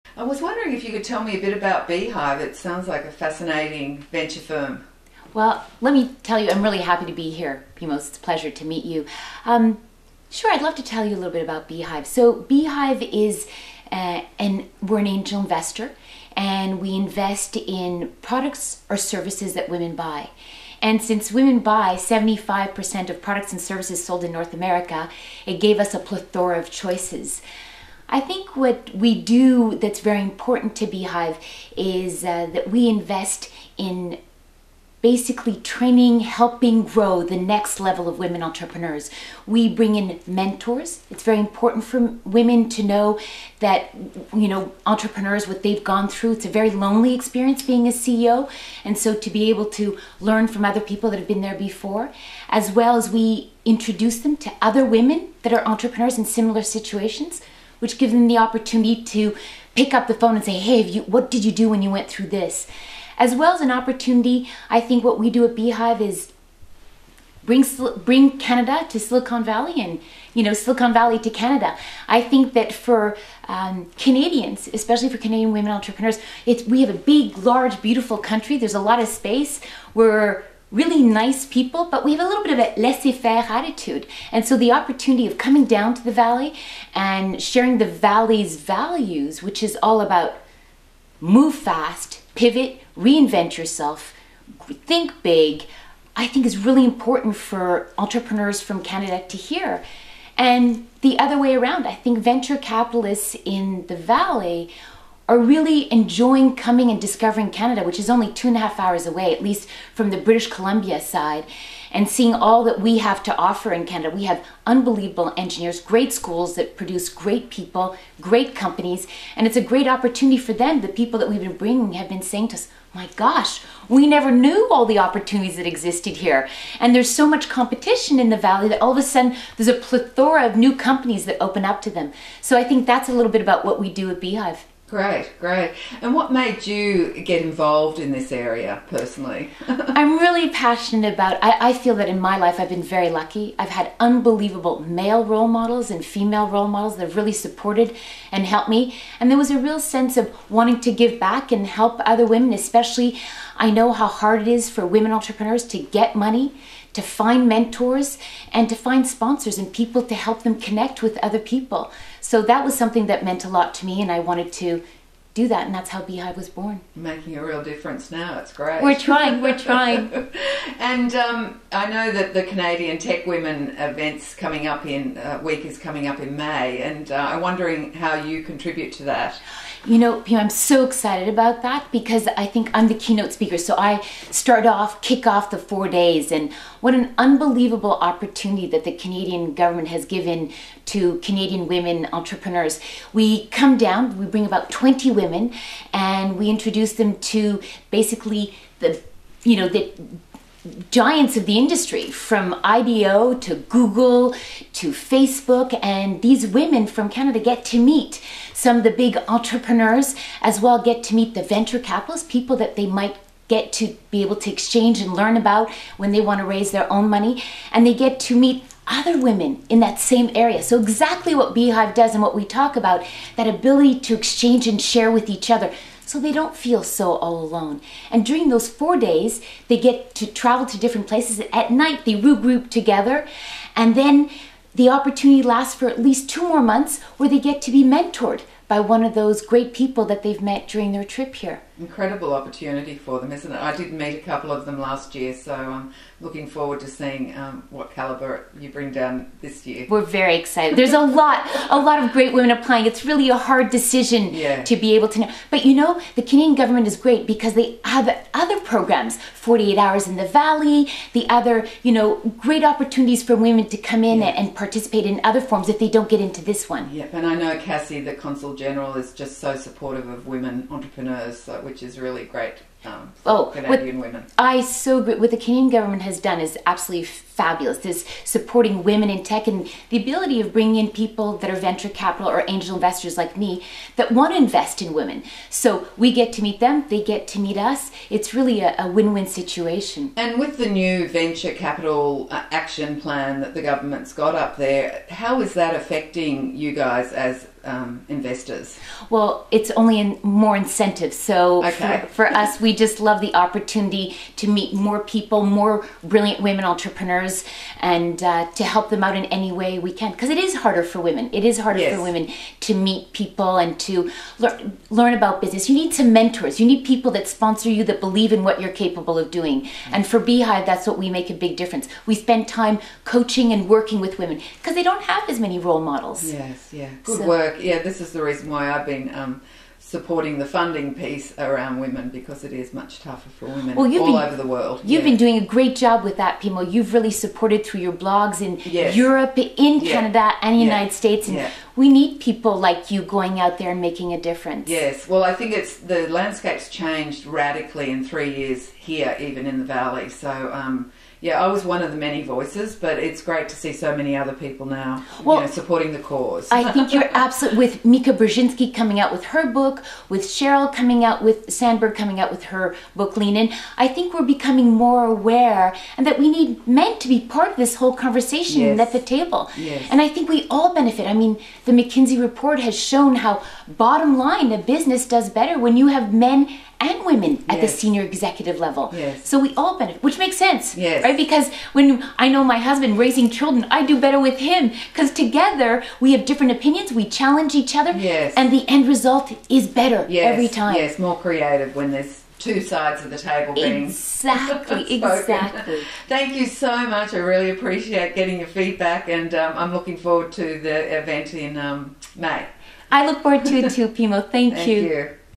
Nanon de Gaspé Beaubien-Mattrick, Beehive: Betting on Female Focused Startups
Nanon de Gaspé Beaubien-Mattrick is President and Founder of Beehive Holdings.  Nanon is responsible for the deal flow in the USA, shaping the strategy and culture of Beehive Holdings, and for overseeing implementation of its strategic plan.  Born in Montreal, QC, Nanon attended a French Lyceé, then headed to Phillips Academy in Andover, MA.  Nanon received a Bachelor of Arts (Honors) from Harvard College and an MBA from Harvard Business School.   Prior to founding Beehive, Nanon worked in sales and marketing for Playtex International before moving on to become Sr. Vice President of Telemedia Corporation for 10 years.  Nanon resides in BC with her husband and 2 children.   Nanon is currently serving as a Director on the following Boards: UBC/VGH Hospital Foundation, Forum for Women Entrepreneurs (FWE), and a foundation dedicated to resolving North America's water crisis and the de Gaspé Beaubien Family Foundation.